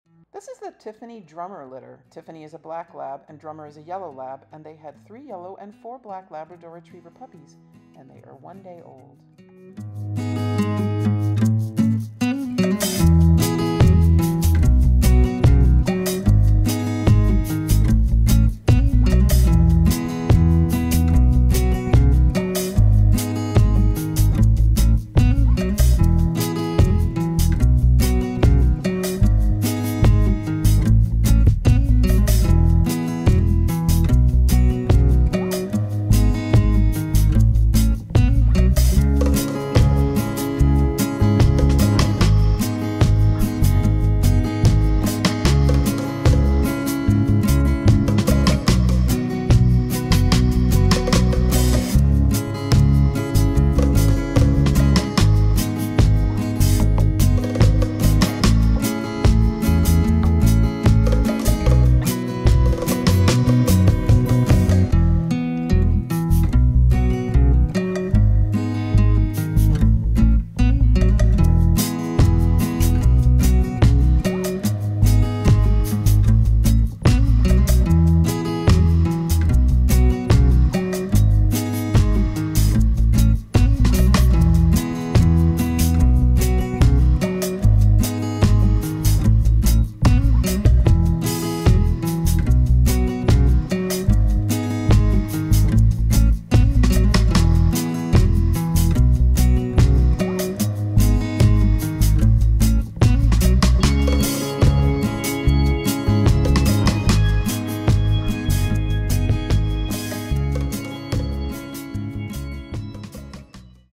3 21 2024 Tiffany
Tiffany x Drummer
3 yellow and 4 black lab puppies
DOB 3/20/2024
1 day old